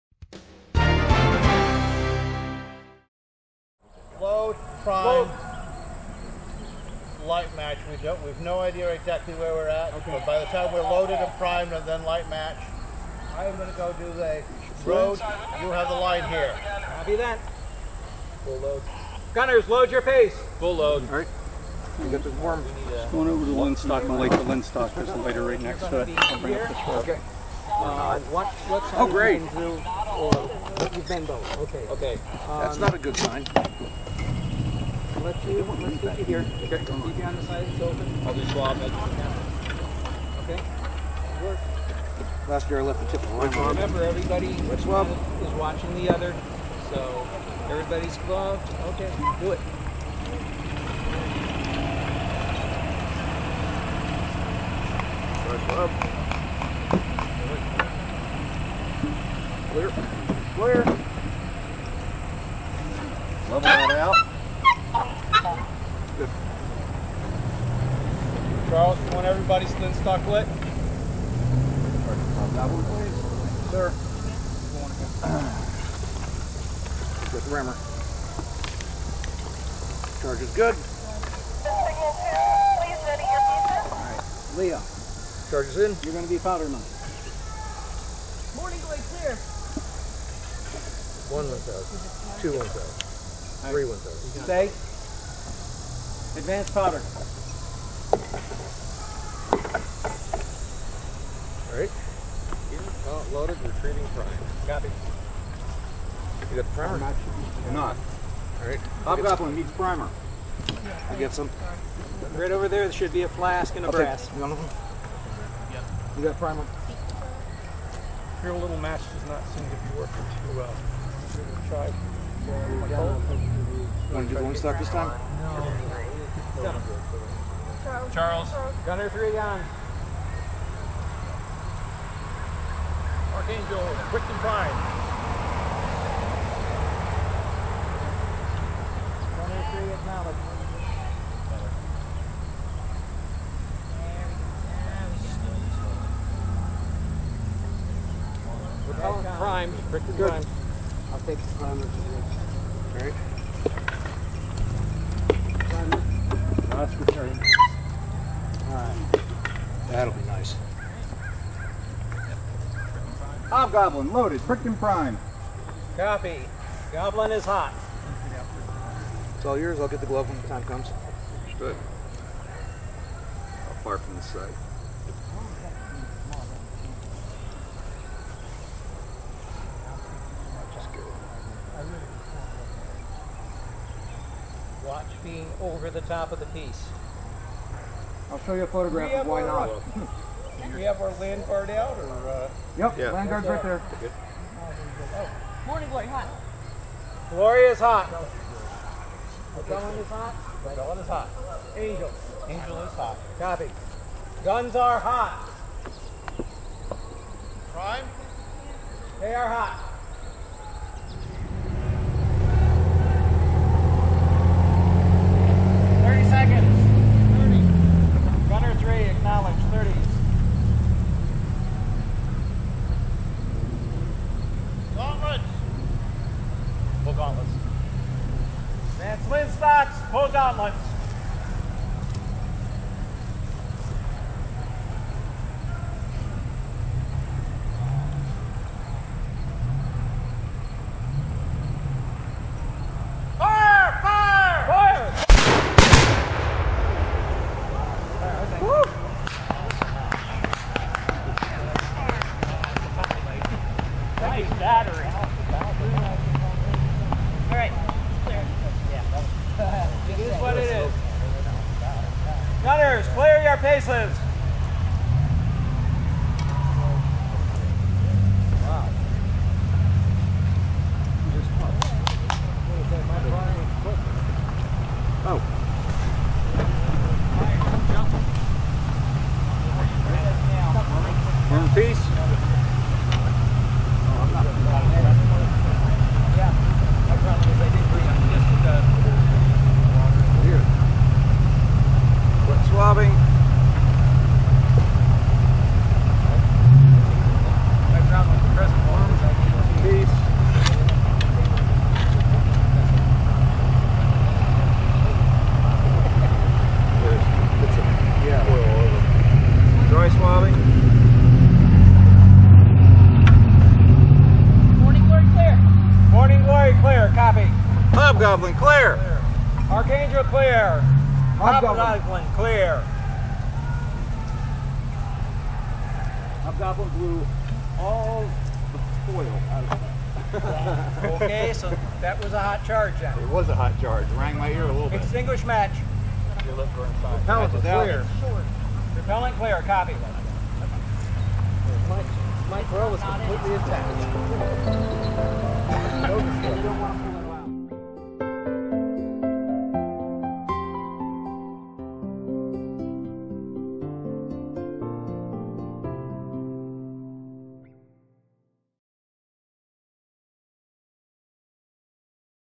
Pennsic 2018 Woods Battle Canon firing Video #1
4 canon battery fired near the beginning of the Woods battle at Pennsic 47 (2018)  This is one of several videos.
Audio and video recording and editing by Emory Stagmer (@VAXHeadroom) of UnTied Music.